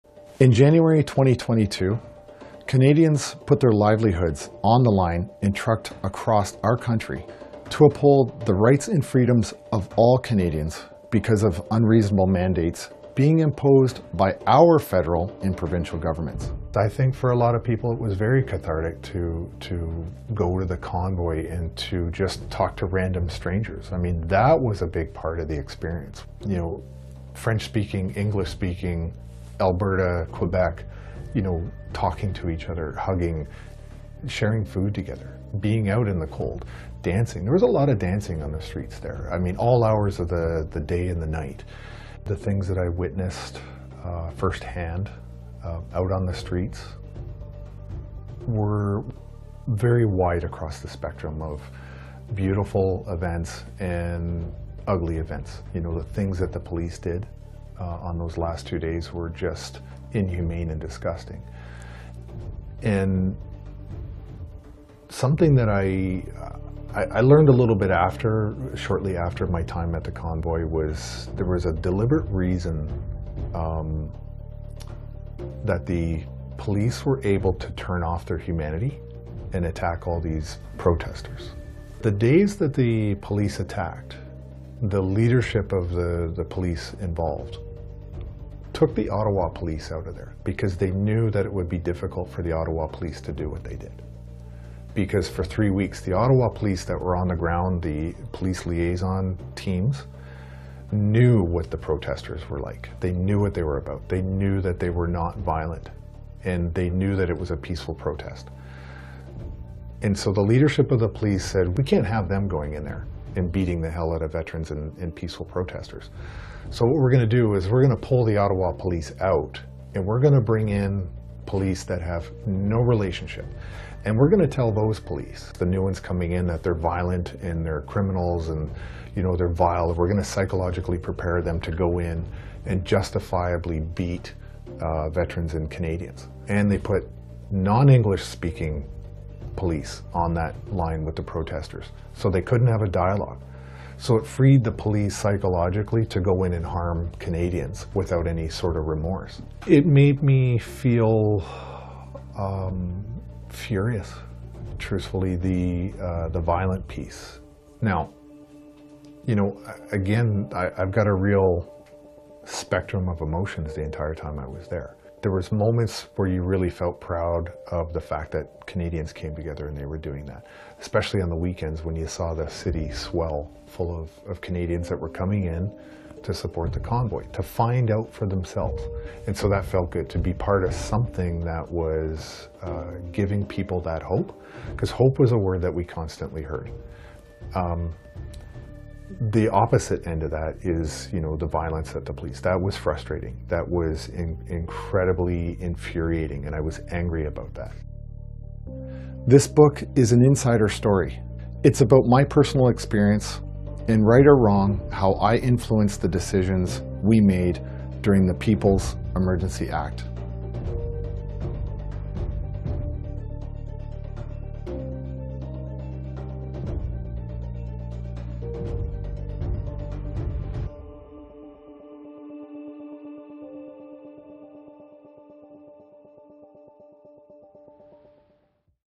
Tom Marazzo Book – The People’s Emergency Act ~ Extreme Violence VS Extraordinary Humanity
It’s OFFICIAL!

“The things that I witnessed firsthand out on the streets were very wide across the spectrum of beautiful events and ugly events. For a lot of people it was very cathartic to just talk to random strangers. That was a big part of the experience. French speaking, English speaking, Alberta, Quebec, all talking to each other and hugging.

The things the police did on those last two days were just inhumane and disgusting. On the days the police attacked the police leadership took the Ottawa police out of there because for three weeks they were on the ground and they knew that we were not violent, and that it was a peaceful protest.”

In January 2022, Canadians put their livelihoods on the line and trucked across our country in the bitter cold to uphold the Rights and Freedoms of ALL Canadians, because of unreasonable mandates being imposed by our Federal and Provincial governments.
This book is an insider story. My account of life events leading up to, during and after the Freedom Convoy.  It’s about my personal experience, and right or wrong, how I influenced the decisions we made during the People’s Emergency Act.
The People’s Emergency Act: Freedom Convoy 2022